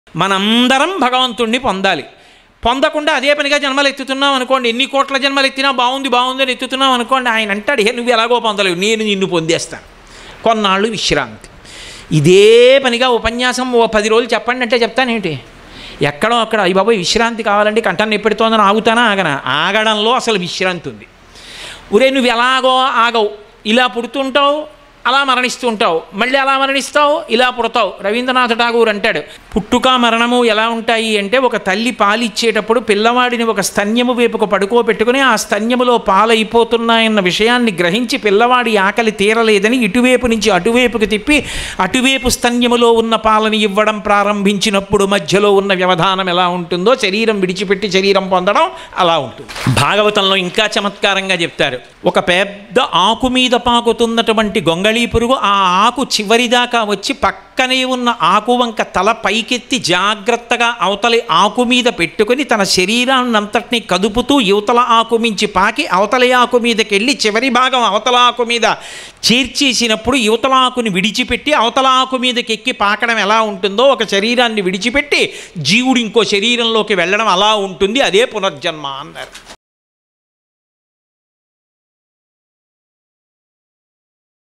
జన్మ పునర్జన్మ మధ్య స్థితి ఎలావుంటుంది? | చాగంటి గారి ప్రవచనం
What is the state between birth and rebirth?

బ్రహ్మశ్రీ చాగంటి కోటేశ్వరరావు గారి ప్రవచనం..  | Brahmasri Chaganti Koteswara Rao gari Pravachanam..